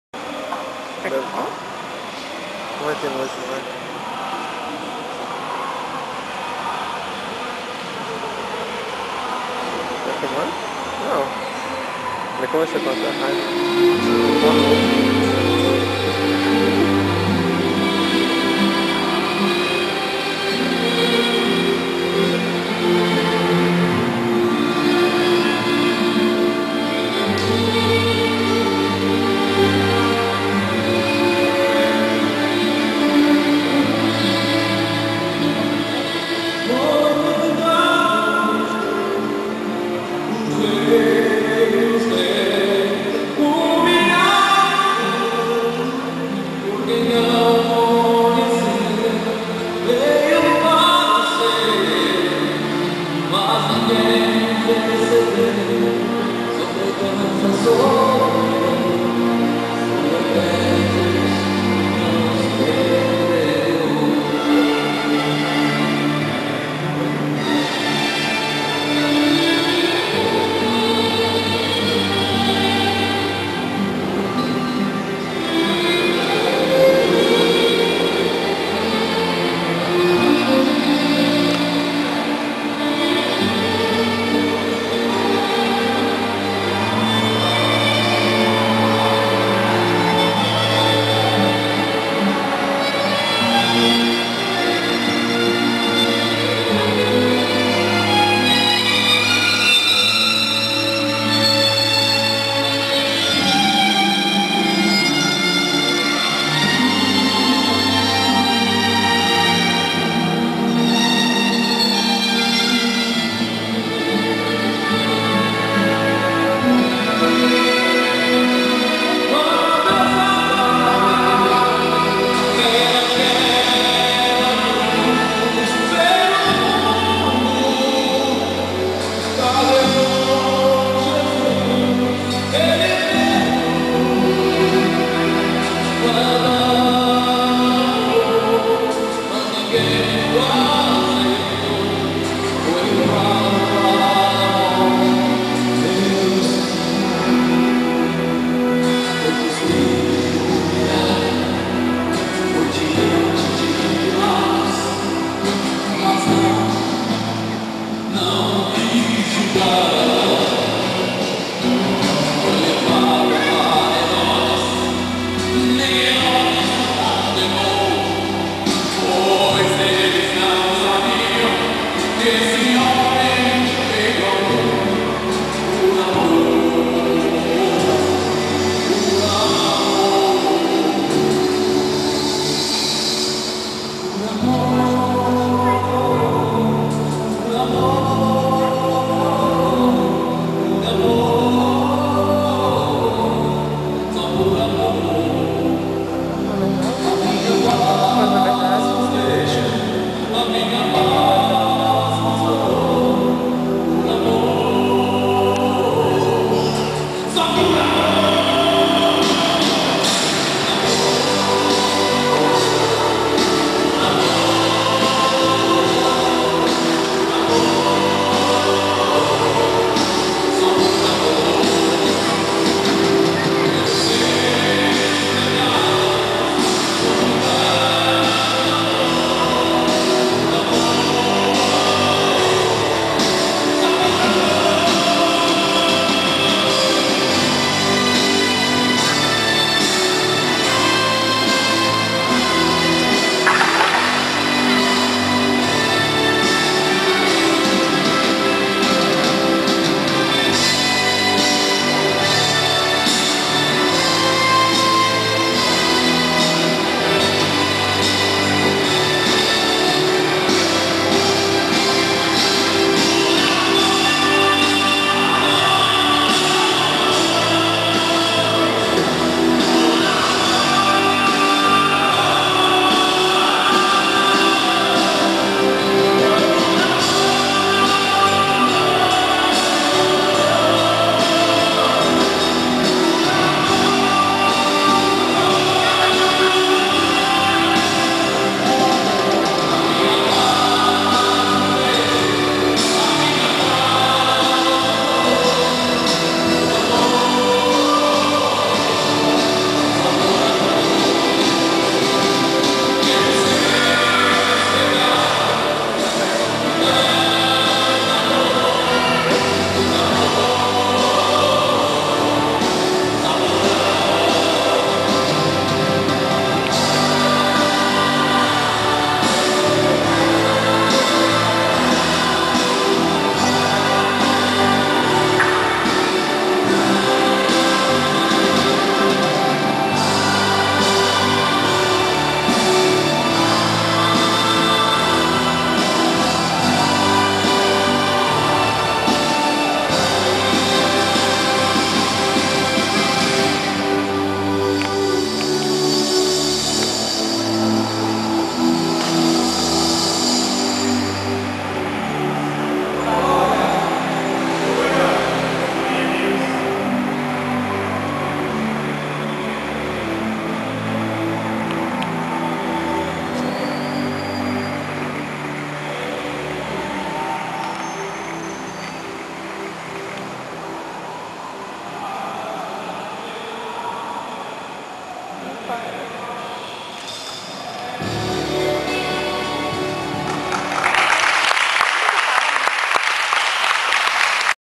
Águia - Teatro Cristão - Na Batista Ame Jesus - Peça: Por Amor
Nossa peça POR AMOR uma Benção, Nós a Criamos debaixo da Direção do Espirito Santo

Veja esta peça, ela é uma Benção

Teatro Águia